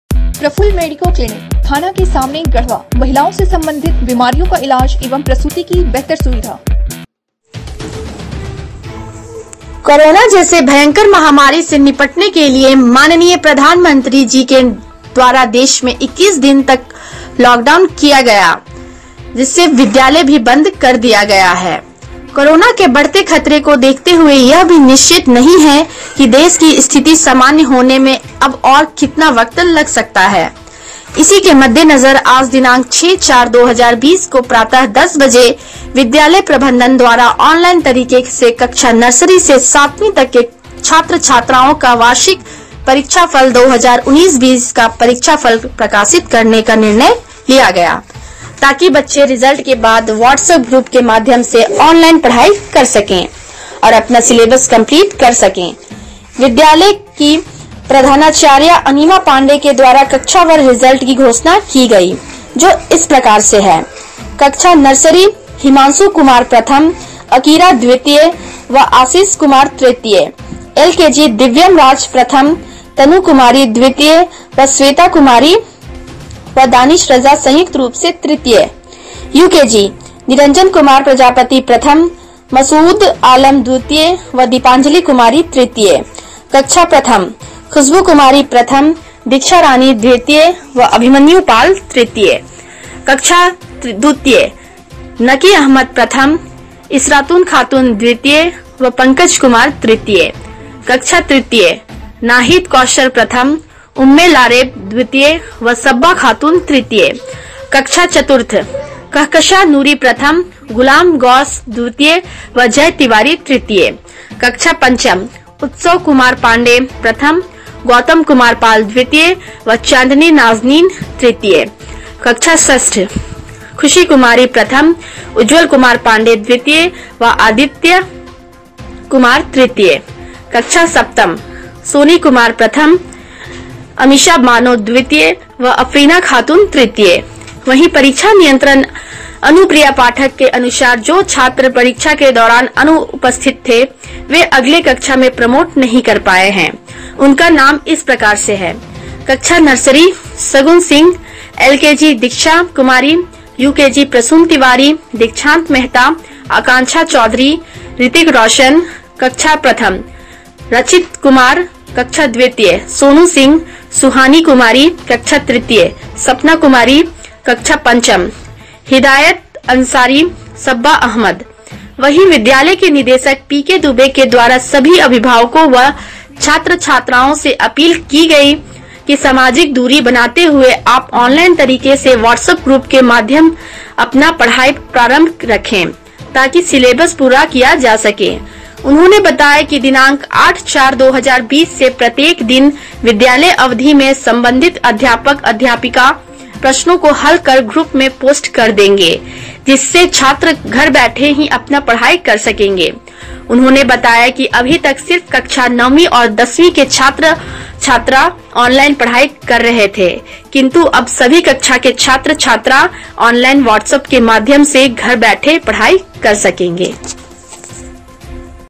गढ़वा। एआरडी स्कूल का परीक्षाफल ऑनलाइन हुआ
कोरोना जैसे भयंकर महामारी से निपटने के लिए माननीय प्रधानमंत्री जी के द्वारा देश मे 21 अप्रैल तक लॉकडाउन किया है।जिससे विद्यालय भी बंद कर दिया गया है।कोरोना के बढ़ते खतरे को देखते हुए यह भी निश्चित नहीं  कि देश की स्थिति सामान्य होने में अभी और कितना वक्त लग सकता है।इसी के मद्देनजर आज दिनांक 06/04/2020 को प्रातः 10 बजे विद्यालय प्रबंधन द्वारा ऑनलाइन तरीके से कक्षा नर्सरी से सातवीं तक के छात्रों का वार्षिक परीक्षा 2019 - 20 का परीक्षा फल प्रकाशित करने का निर्णय लिया गया। ताकि बच्चे रिजल्ट के बाद वाट्सअप ग्रुप के माध्यम से ऑनलाइन पढ़ाई कर सकें और अपना सिलेबस कम्प्लीट कर सकें।विद्यालय की प्रधानाचार्या अणिमा पांडेय के द्वारा कक्षावार रिजल्ट की घोषणा की गई जो इस प्रकार है।
कक्षा नर्सरी: हिमांशु कुमार प्रथम, अकीरा द्वितीय व आशिस कुमार तृतीय। एलकेजी : - दिव्यम राज प्रथम, तनु कुमारी द्वितीय व स्वेता कुमारी व दानिश राजा संयुक्त रूप से तृतीय।यूकेजी : - निरंजन कुमार प्रजापति प्रथम, मसऊद आलम द्वितीय व दीपांजलि कुमारी तृतीय।कक्षा प्रथम : खुशबू कुमारी प्रथम, दीक्षा रानी द्वितीय व अभिमन्यु पाल तृतीय।कक्षा द्वितीय: - नकी अहमद प्रथम, इशरातून खातून द्वितीय व पंकज कुमार तृतीय।कक्षा तृतीय: - नाहिद कौसर प्रथम, उम्मे लारेब द्वितीय व सब्बा खातून तृतीय।कक्षा चतुर्थ : - कहकशा नूरी प्रथम, गुलाम गौस द्वितीय व जय तिवारी तृतीय।कक्षा पंचम: - उत्सव कुमार पांडेय प्रथम, गौतम कुमार पाल द्वितीय व चांदनी नाजनीन तृतीय।कक्षा षष्ट: - खुशी कुमारी प्रथम, उज्ज्वल कुमार पांडेय द्वितीय व आदित्या कुमार तृतीय।कक्षा सप्तम : - सोनी कुमारी प्रथम, अमीषा बानो द्वितीय व अफ्रीना खातून तृतीय।
         वहीं परीक्षा नियंत्रक अनुप्रिया पाठक के अनुसार जो छात्र परीक्षा के दौरान अनुपस्थित थे वे अगले कक्षा में प्रमोट नहीं कर पाए हैं उनका नाम इस प्रकार है।
  कक्षा नर्सरी: - सगुन सिंह,   एलकेजी: - दीक्षा कुमारी,
यूकेजी: - प्रसून तिवारी,दीक्षांत मेहता,आकांक्षा चौधरी,रितिक रौशन,
कक्षा प्रथम: - रचित कुमार,
कक्षा द्वितीय: - सानू सिंह, सुहानी कुमारी,
कक्षा तृतीय: - सपना कुमारी,
कक्षा पंचम: - हिदायत अंसारी,सब्बा अहमद।
         वहीं विद्यालय के निदेशक पी के दुबे के द्वारा सभी अभिभावकों व छात्र छात्राओं से अपील की गई की सामाजिक दूरी बनाते हुए आप ऑनलाइन तरीके से वाट्सअप ग्रुप के माध्यम अपना पढ़ाई प्रारम्भ रखें।ताकि सिलेबस पूरा किया जा सके। उन्होंने बताया कि दिनांक 8/04/2020 से प्रत्येक दिन विद्यालय अवधि में सम्बन्धित अध्यापक/अध्यापिका प्रश्नों को हल कर ग्रुप में पोस्ट कर देंगे ।जिससे छात्र घर बैठे ही अपना पढ़ाई कर सकेंगे।उन्होंने बताया कि अभी तक सिर्फ कक्षा नवीं और दसवीं के छात्र ऑनलाइन पढ़ाई कररहे थे किंतु अब सभी कक्षा के छात्र ऑनलाइन वाट्सअप के माध्यम से घर बैठे पढ़ाई कर सकेंगे।

Talash TV
Latest News
Breaking News
Current News
garhwa news
palamu news
latehar news